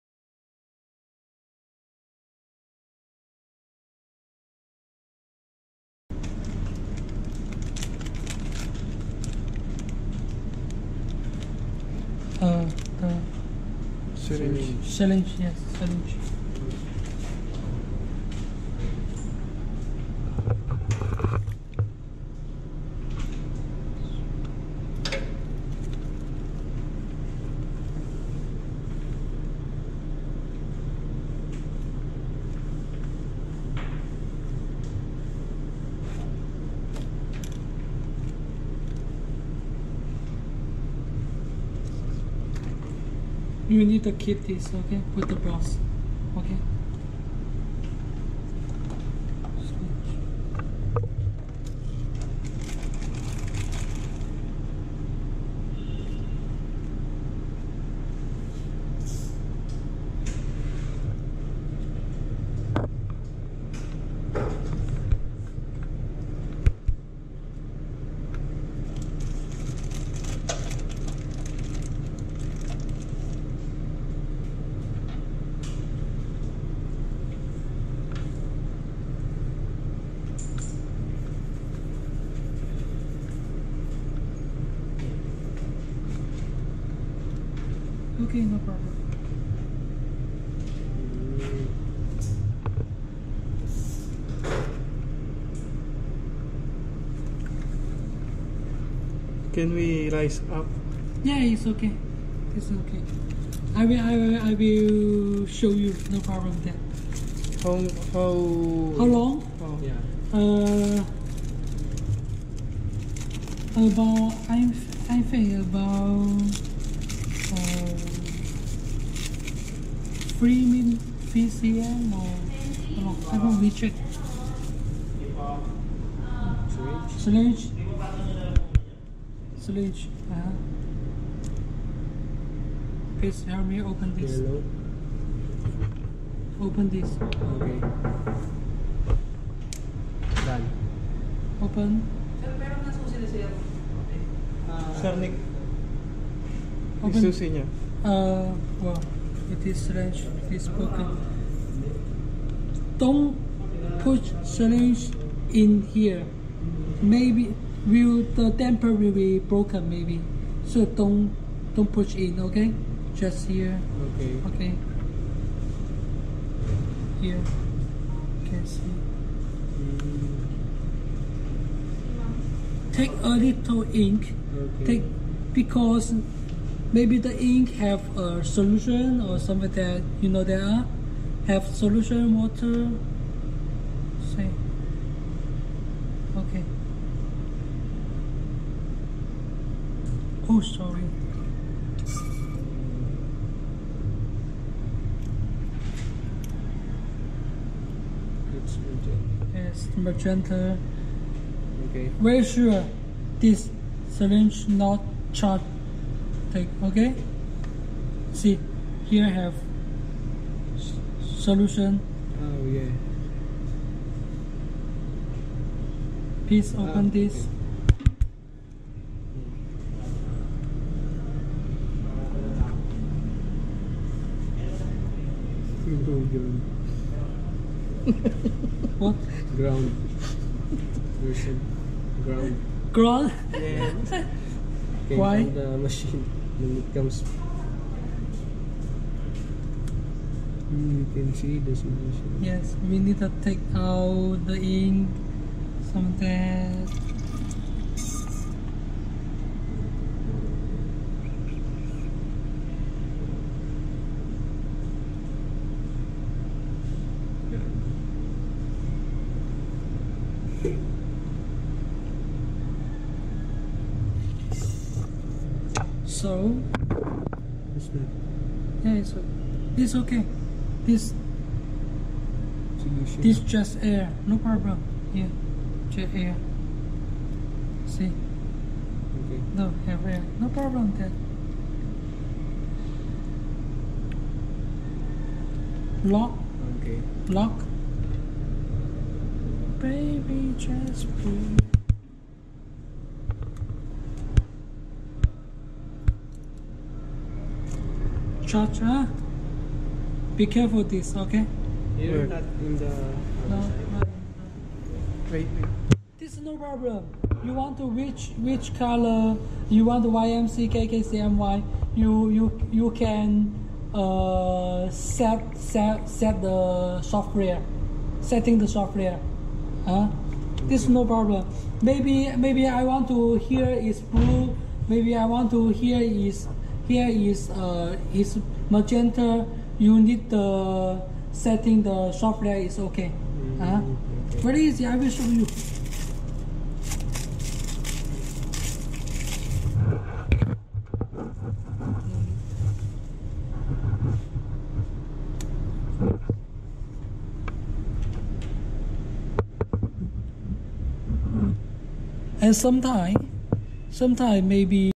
How to suck ink and install ink damper and head- Xaar 1201
This video is about how to such ink and install ink damper for Dika and Xuli printers with Xaar 1201 heads.